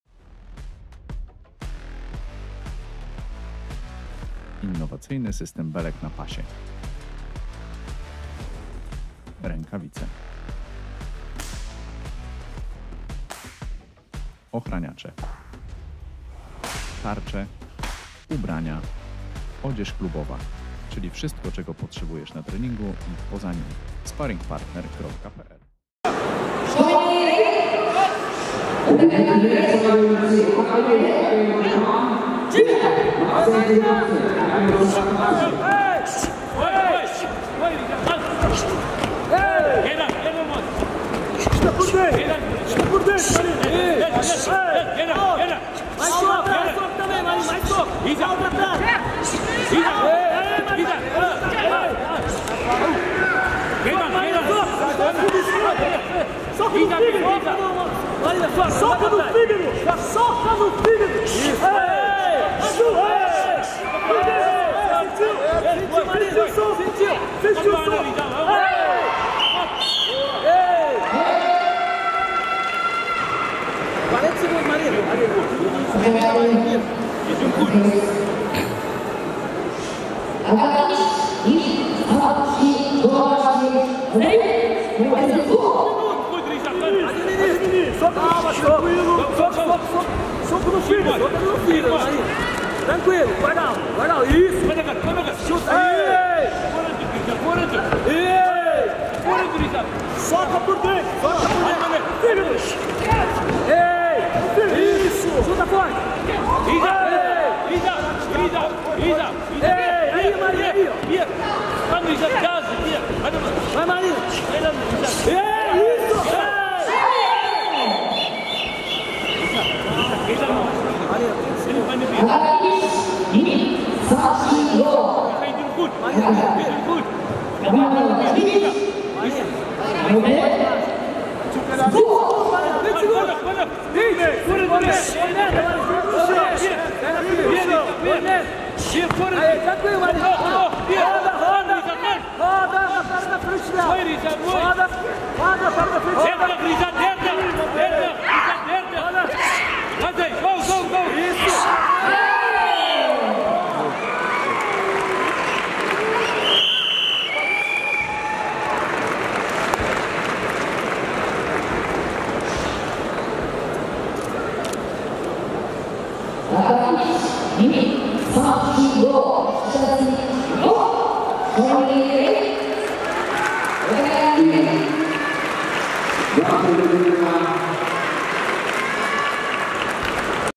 Riza Rootman vs Marina Issa | WORLD TEAM MATCHES Junior Girl Open | IKO WORLD CUP KATOWICE 2024
Jeśli organizujesz turniej lub inne wydarzenie sztuk walki i potrzebujesz nagrań takich jak to, to zapraszamy do kontaktu mailowego.

IKO World Cup, 21st European Open, European Cup, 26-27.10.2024, Katowice